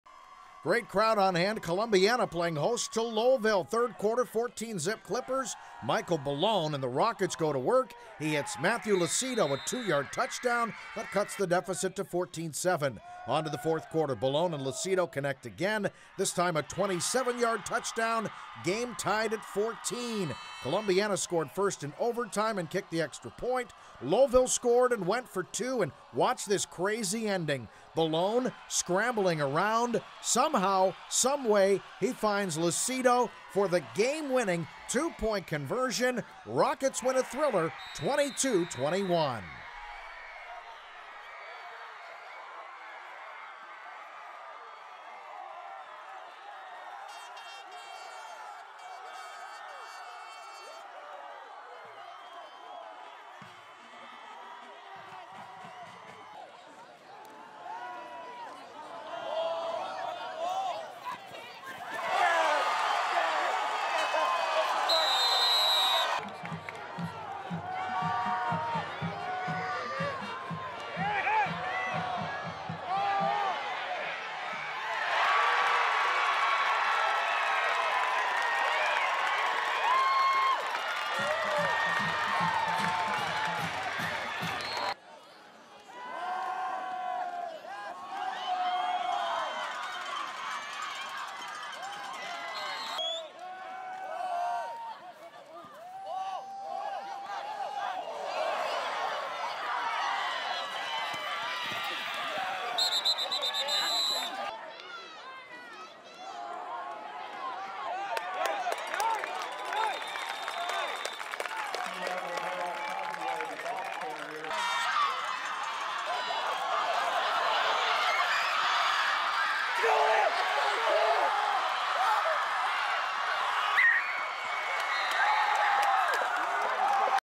Lowellville Rockets vs. Columbiana Clippers high school football
Lowellville stuns Columbiana on final play.